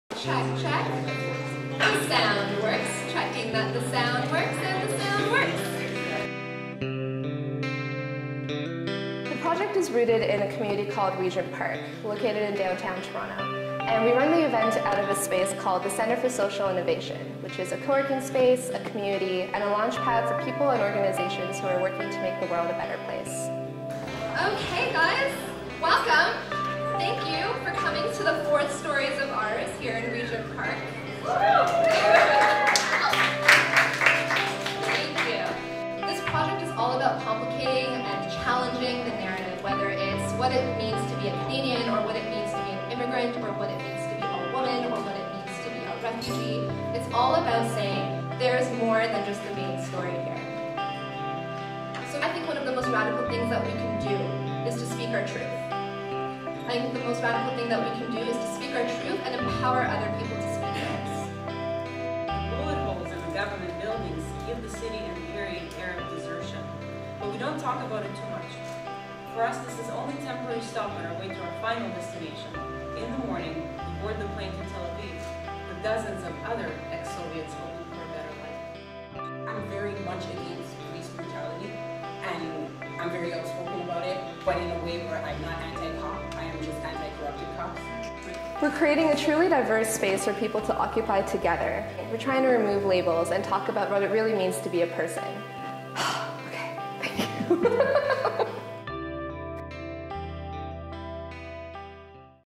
Stories of Ours: Challenging Dominant Narratives through Stories and Art
Stories of Ours is a project that aims to deepen community, invite solidarity, and challenge dominant narratives through intentional acts of storytelling.

Original music by Sare D'Alimonte